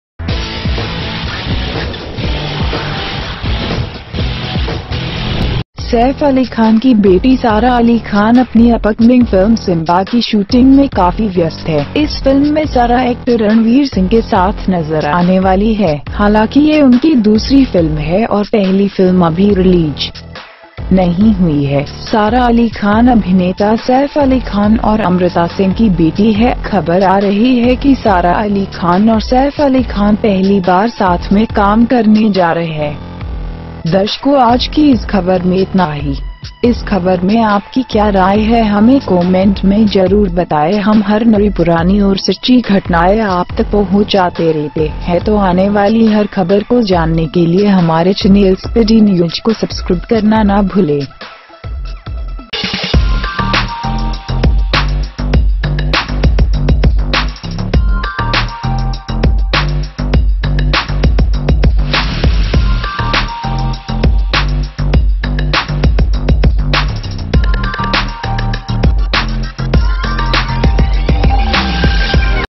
रियल लाइफ बाप बेटी रील लाइफ में भी बाप बेटी का रोल करते हुए आएंगे नजर
Saif Ali Khan's daughter, Sarah Ali Khan, is very busy shooting for her upcoming movie, Sibaba. The film is going to be seen with all the actor Ranvir Singh. Although this is his second film and the first film is not yet released. Sarah Ali Khan is the daughter of actor Saif Ali Khan and Amrita Singh. The news is that Sara Ali Khan and Saif Ali Khan are going to work together for the first time. The funny thing is that in this film, Real Life is a father-daughter roller in father-daughter reel life too.